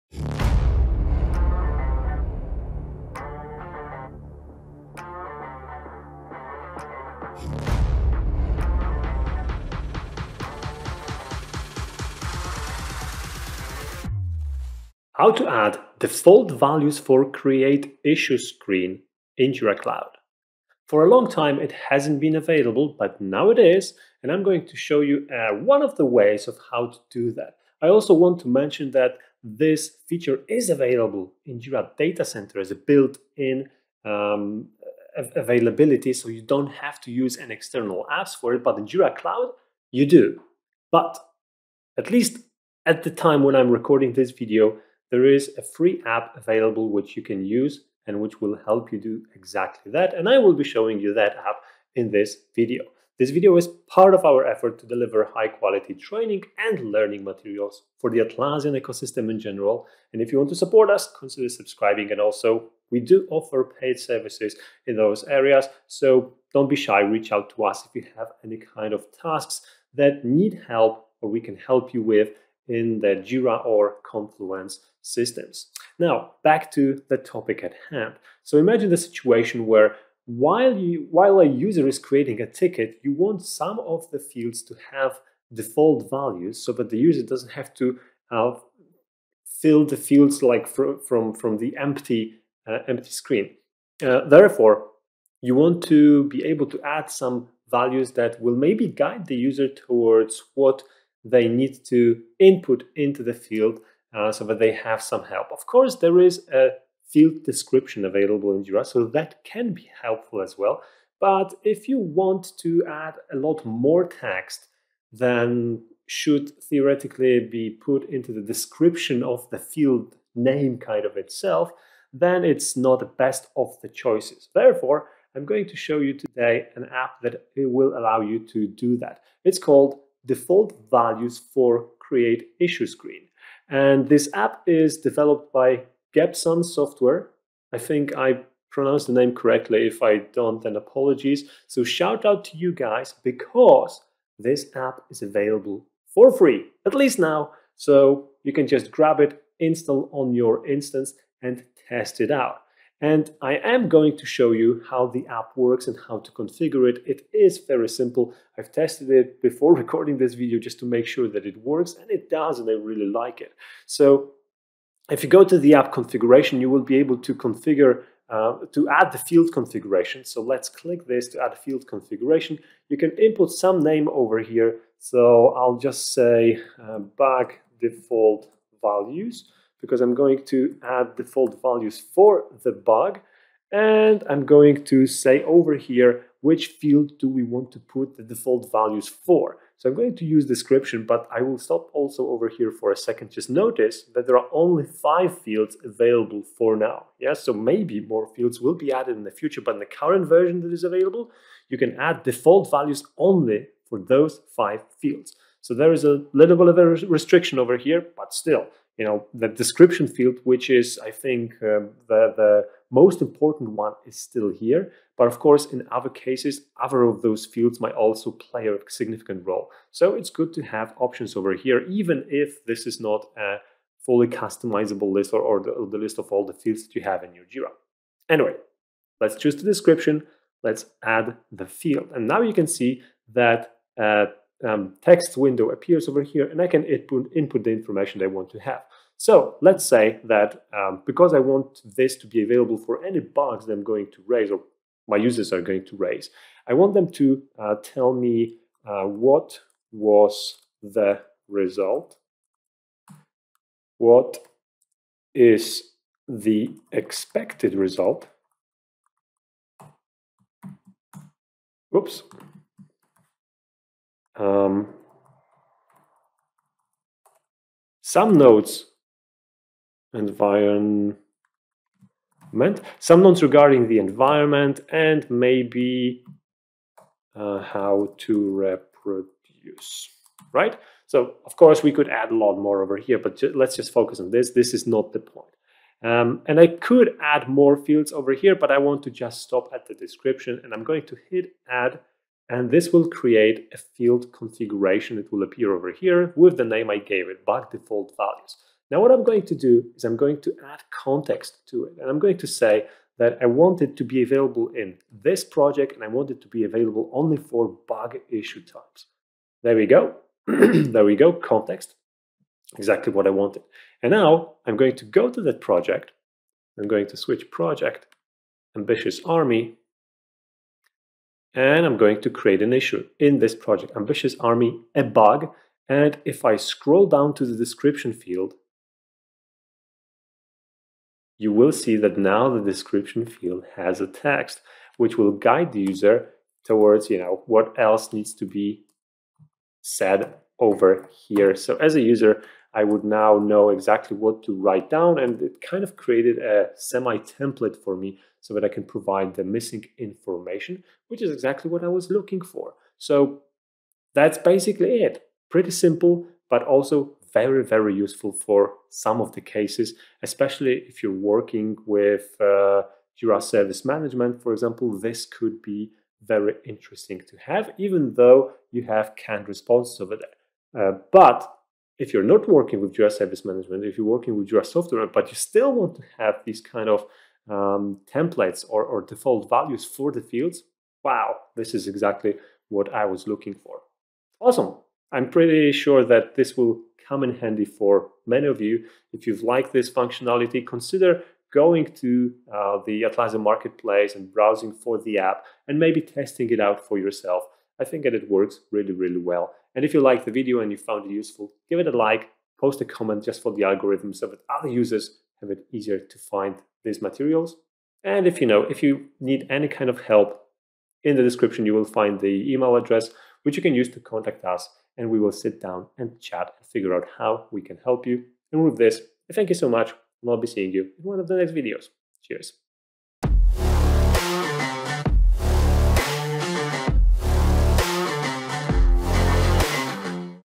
Adding Default Field Values to Jira Issues Made Simple!
Streamline your issue creation process in Jira with ease! Discover how to add default field values, saving time and improving consistency. Learn step-by-step configuration. Simplify your workflow and boost efficiency. Watch now and unlock the power of default field values in Jira!

Save yourself the trouble of discovering all the pitfalls or non-standard use cases Atlassian ecosystem.